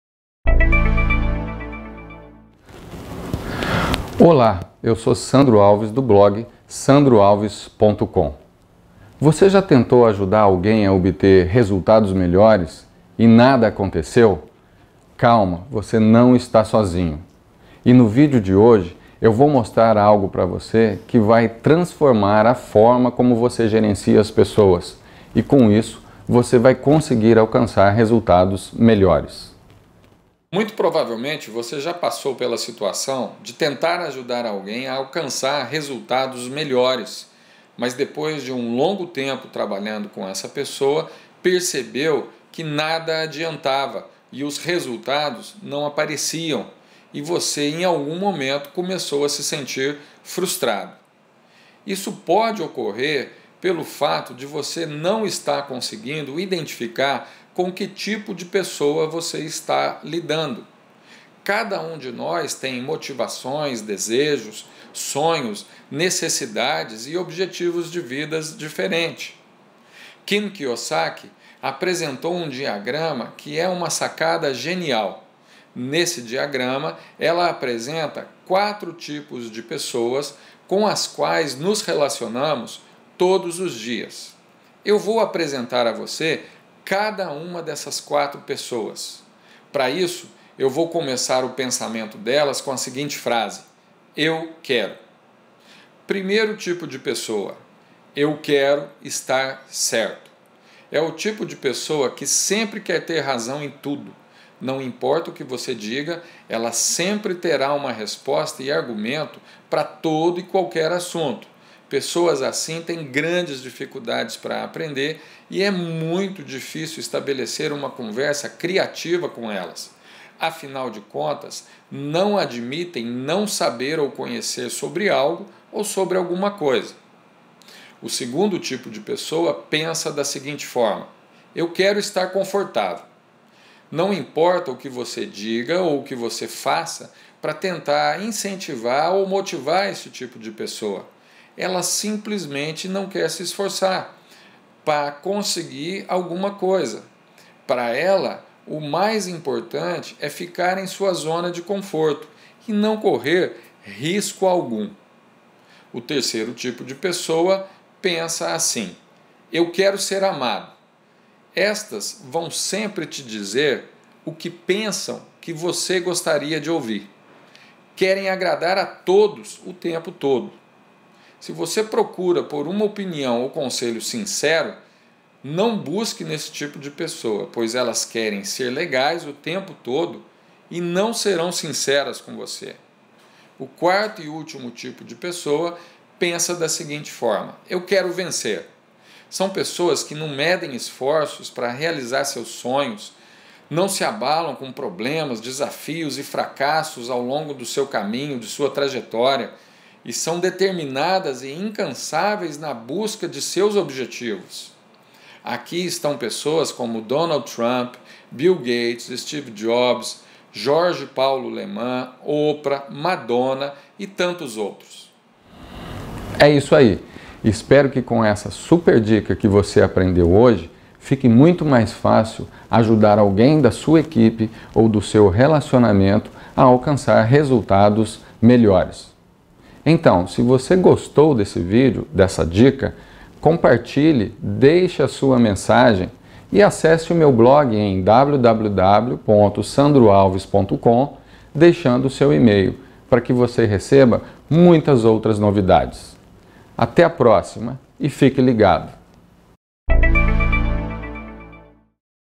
Os 4 Tipos de Pessoas
Aprenda a identificar com que tipo de pessoa você está trabalhando e alavanque os seus resultados.

Você pode estar trabalhando certo, mas com a Pessoa errada num dado Projeto. Com isso, sua expectativa em relação aos resultados que essa pessoa vai te entregar está desalinhado com a realidade - sua expectativa é maior do que aquilo que a pessoa pode ou quer fazer.

Então, saber identificar os tipos de pessoas vai lhe ajudar a gerenciar melhor sua equipe pois saberá delegar tarefas e responsabilidades de forma mais efetiva.

Amplie Suas Possibilidades e Transforme Seus Negócios.

Fique Ligado !